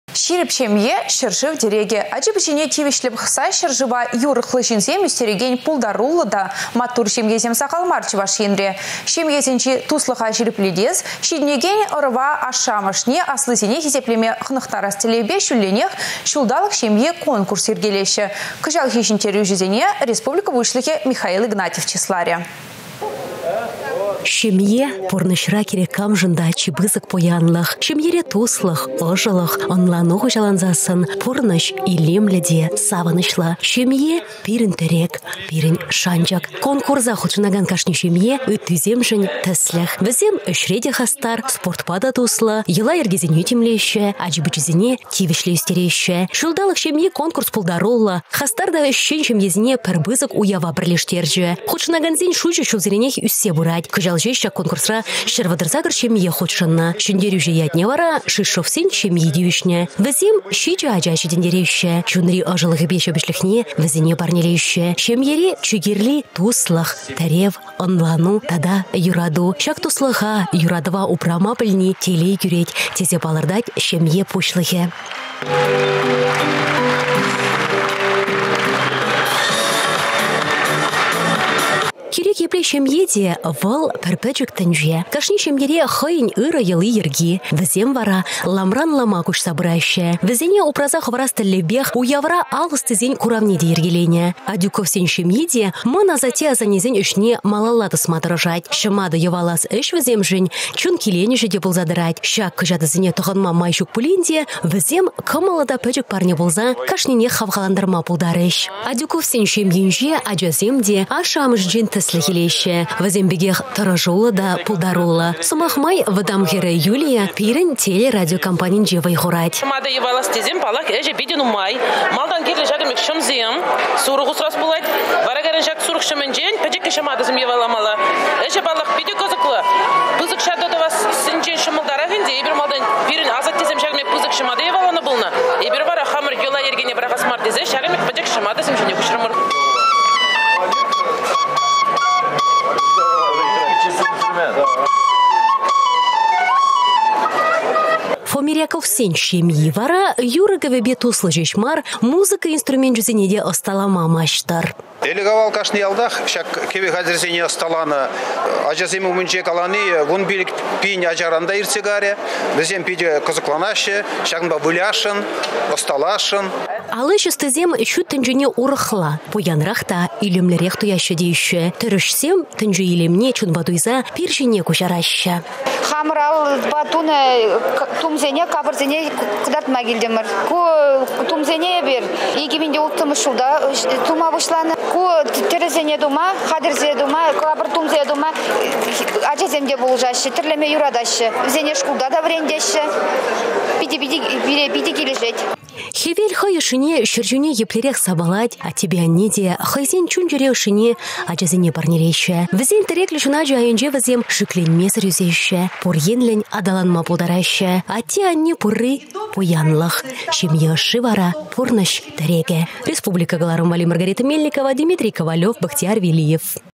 «Çулталăк çемйи»
Çирĕп çемье — çĕршыв тĕрекĕ. Ачи-пăчине тивĕçлĕ пăхса, çĕр-шыва юрăхлă çынсем ÿстрекен пултаруллă та маттур çемьесем сахал мар Чăваш Енре.  Çемьесенчи туслăха çирĕплетес, çитĕнекен ăрăва ашшĕ-амăшне, аслисене хисеплеме хăнăхтарас тĕллевпе çулленех «Çулталăк çемйи» конкурс йĕркелеççĕ